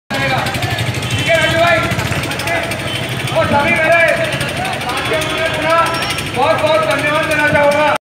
DAILY NEWS MP
इंदौर में करणी सेना  का जमकर विरोध कारकार्ताओ ने किया जमकर विरोध प्रदर्शन